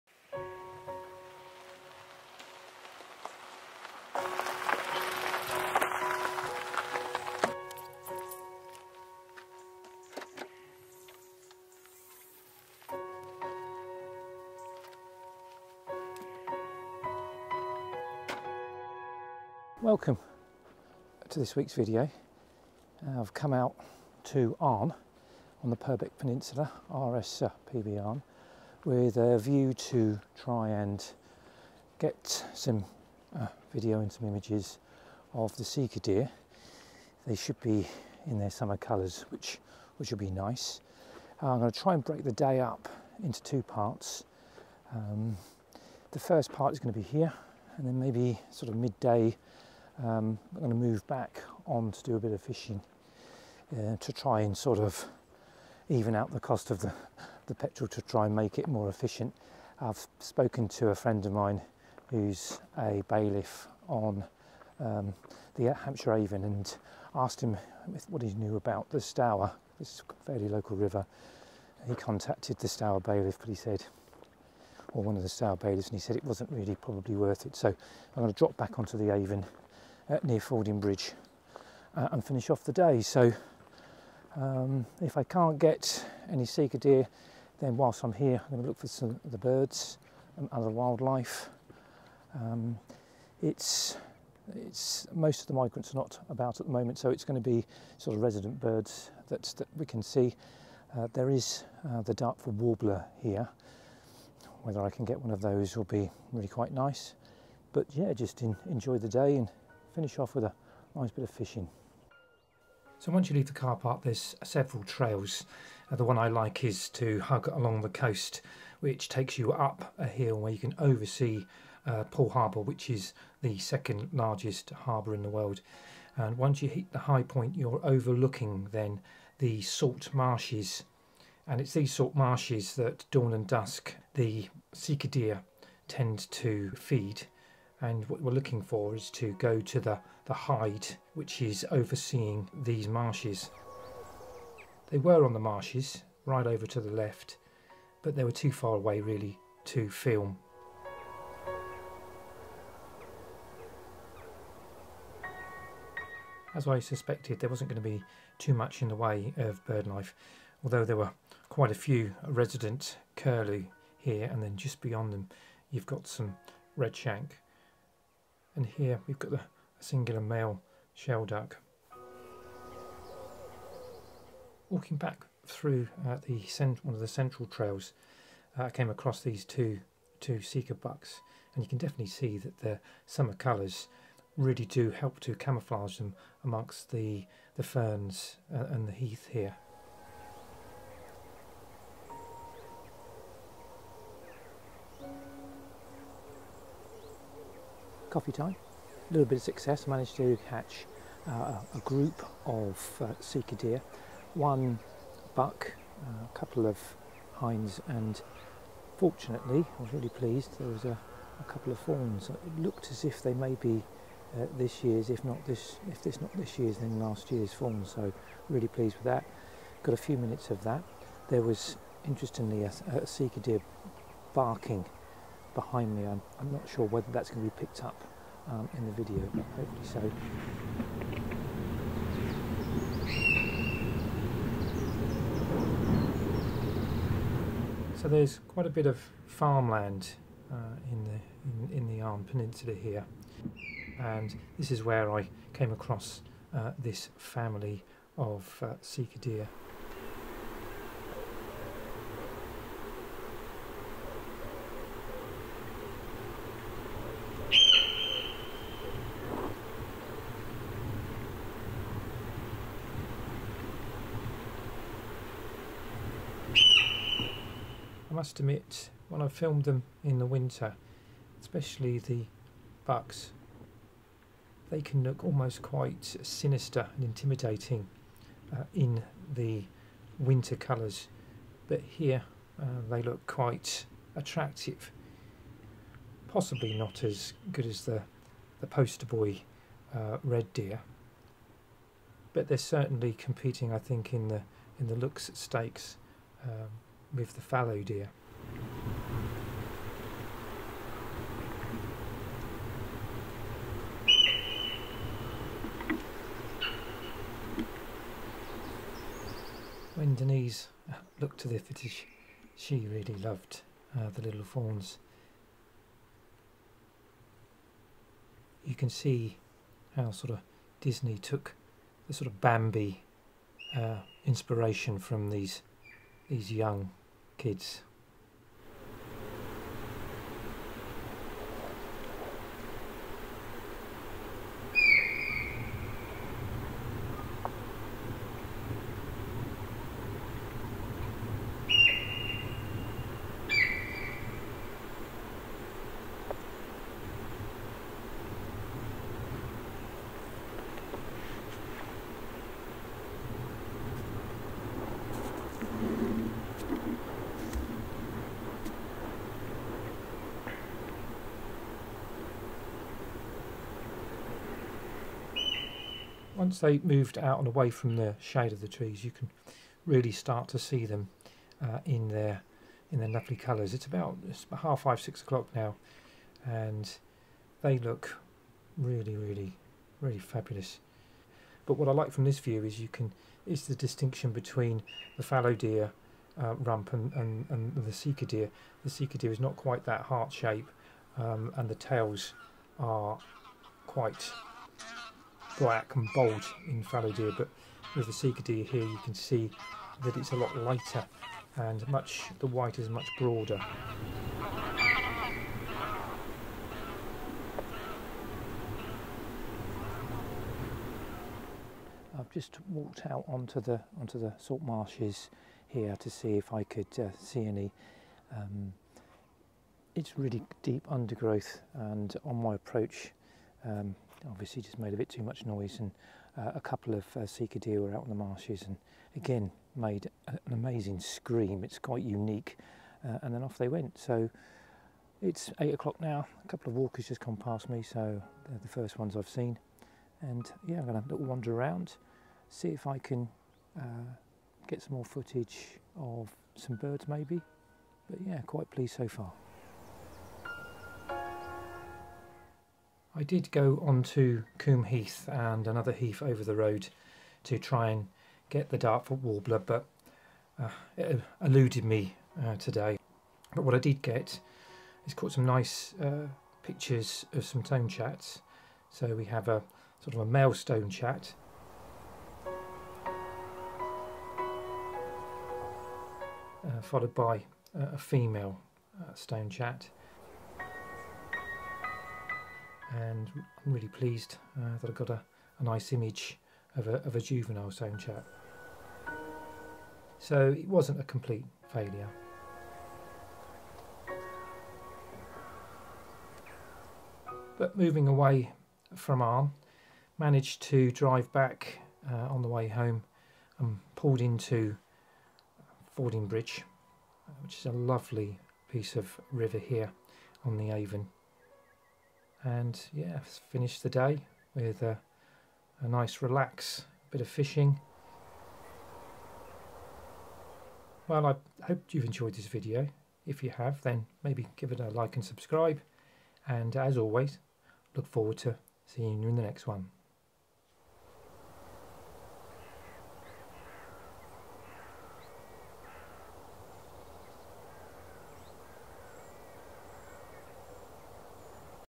Sika Deer at RSPB Arne
A trip to the Arne Nature Reserve. A very early start rewarded by seeing the sika deer in their beautiful summer coats.